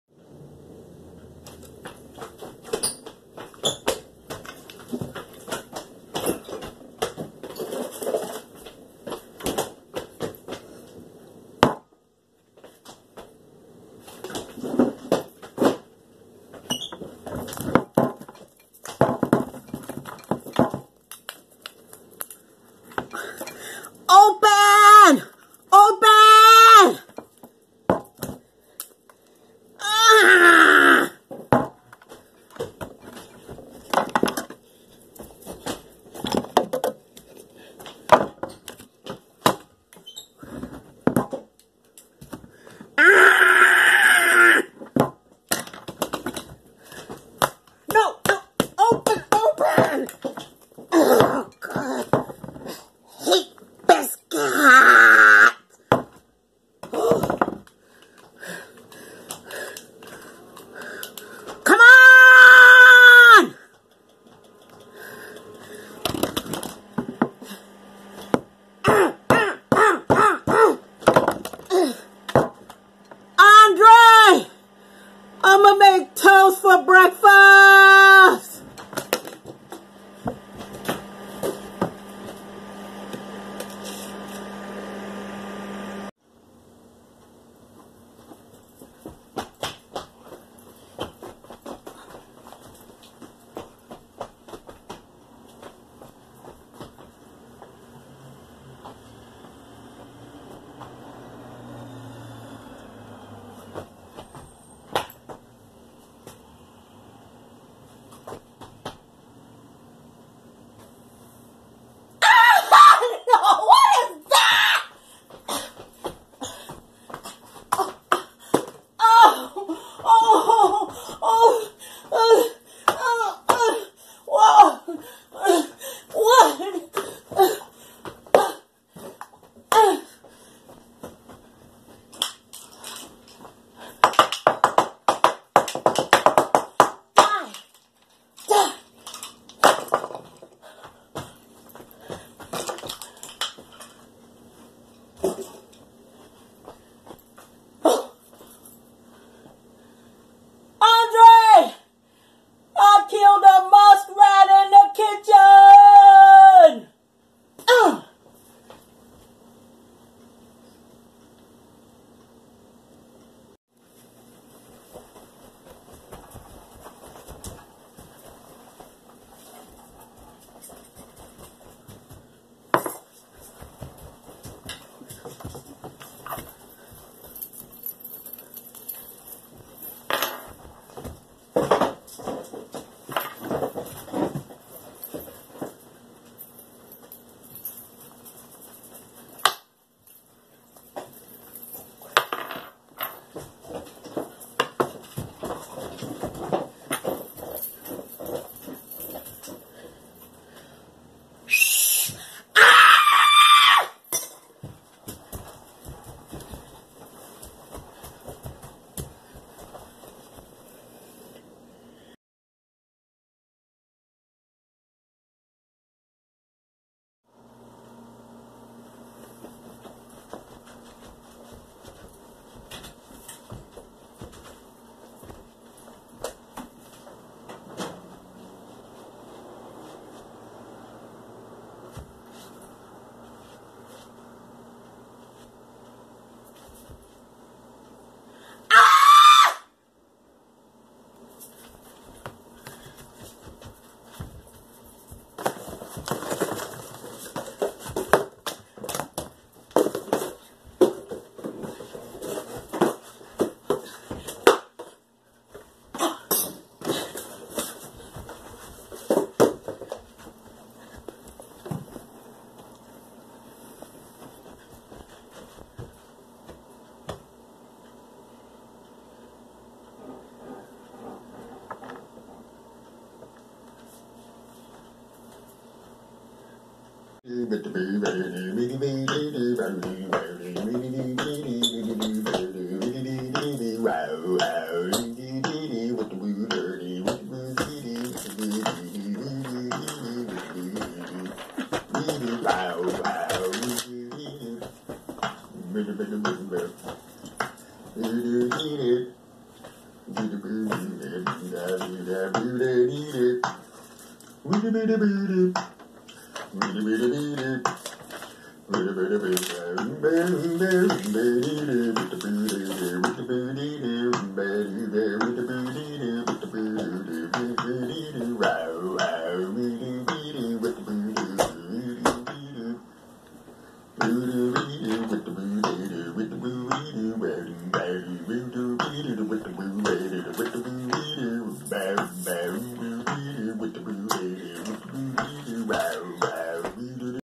Andre's Grandma - Black Table Squawk
Andre's Grandma works out her issues with biscuits, a wig, a whistle and
balls in a box. Then, she wishes everyone a Happy Valentine's Day. All at her black kitchen table. Black Table Squawk. Mouf Muzik© in Happy Valentine's video by Percy Smith. Andre's Grandma played by Marilyn Harris.